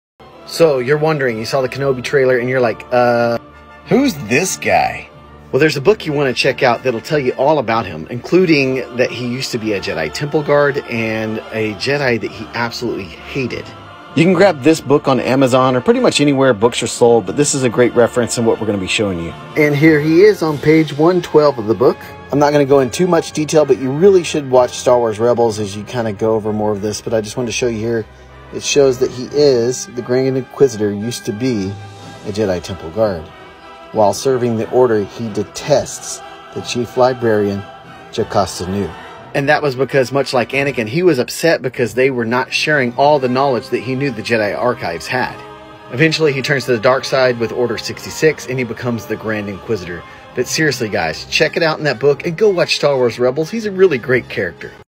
Kenobi Trailer: Who is the Grand Inquisitor?
Giving a little bit of information from the Kenobi Trailer. The Grand Inquisitor. Who is he?

🔹 Help support MHG. Buy us a beer.

🔹Support MHG with our EPIC store creator code🔹
Use code: MULEHORN-GAMING